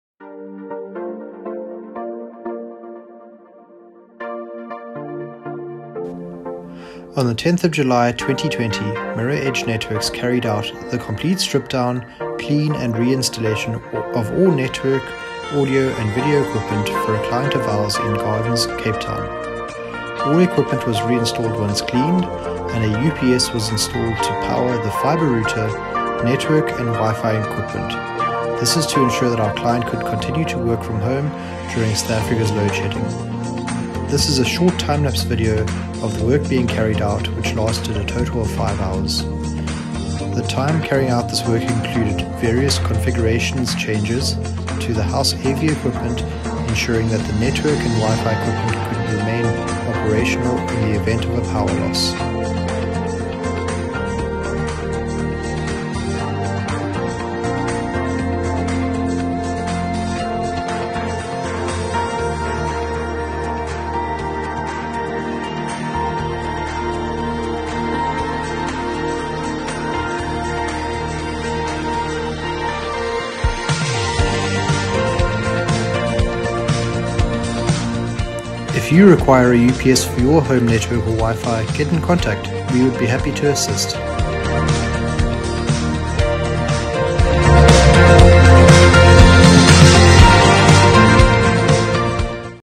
Cape Town UPS installation for Network & WiFi while working from home during Load Shedding
On the 10th of July 2020 Mirror Edge Networks carried out the complete strip down, clean and reinstallation of all network audio and video equipment for a client of ours in Gardens, Cape Town.
All equipment was reinstalled once cleaned.

A UPS was installed to power the fibre router, network and WiFi equipment to ensure that our client could continue to work from home during South Africa’s load shedding.

This is a short time lapse video of the work being carried out which lasted 5 hours in total.
The time carrying out this work included various configurations changes to the house AV equipment and ensuring that the network and WiFi equipment would remain operational in the event of a power loss.

If you require a UPS solution for your home network, get in contact. We would be happy to assist.